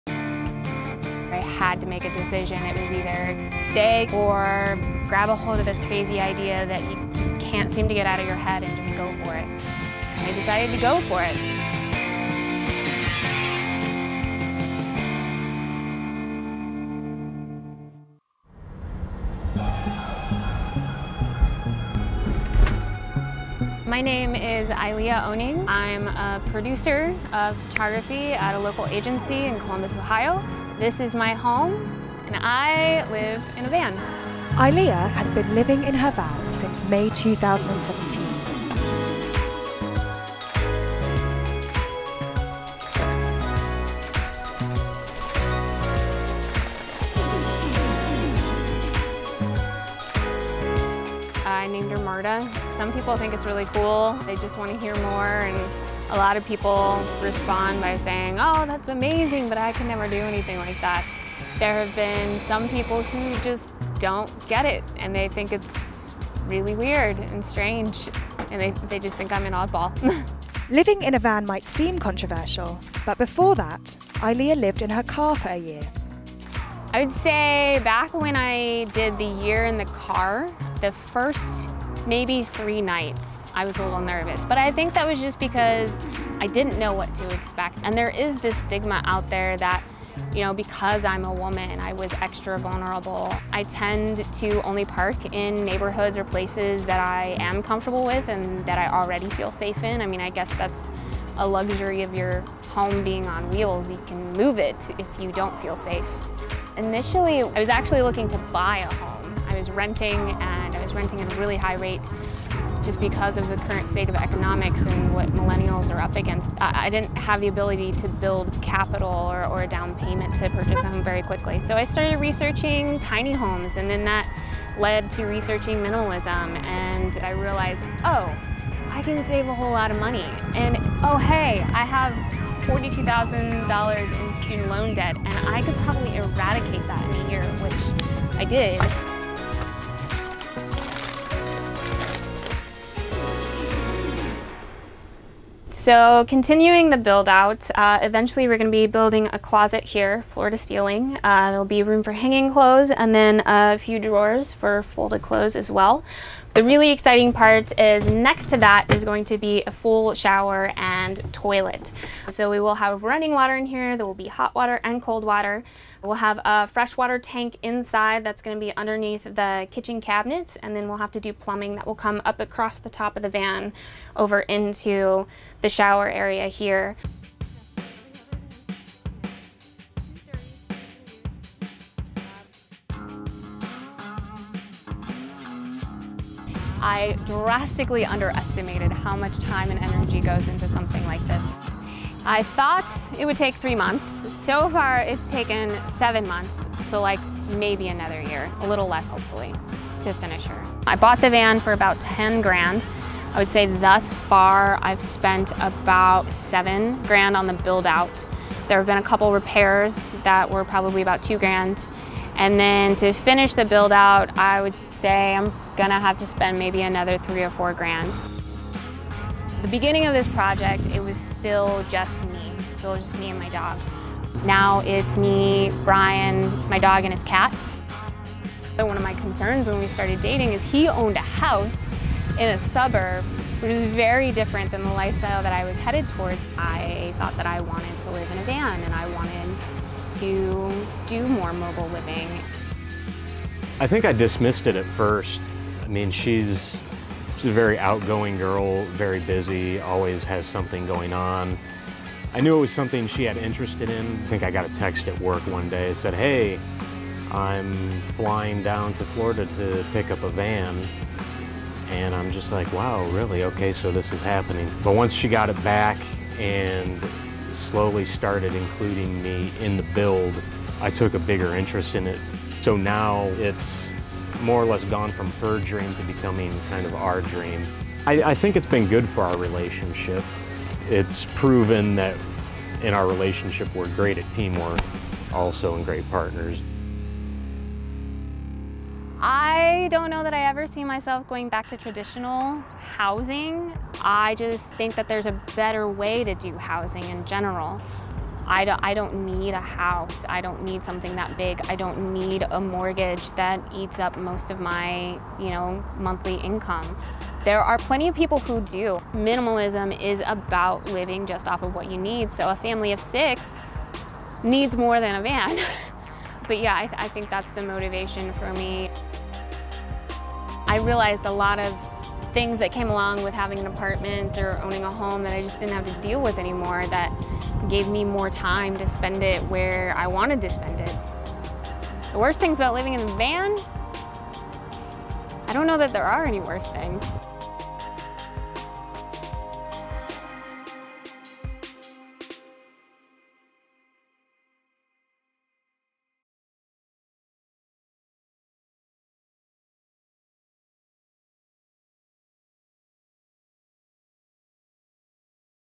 Woman Quits Expensive Rents To Live In A Van
A 31-year-old woman has turned her back on expensive rents and property prices - by living full time in a van. With an interior measuring just 13ft 2in long, 5ft 8in wide and 6ft 2in high, Eileah Ohning’s home is her Freightliner Sprinter High Top van. The photographic producer from Columbus, Ohio, has lived in her compact four-wheel home since May 2017. Complete with a memory foam mattress, storage compartments, a desk and a camping stove, she even has plans to add in a shower, toilet and fridge. Eileah parks her van close enough to her workplace that she never needs to worry about the morning commute and showers at her local gym.

Videographer / director: Adam Lee
Producer: Nathalie Bonney, Ruby Coote
Editor: Marcus Cooper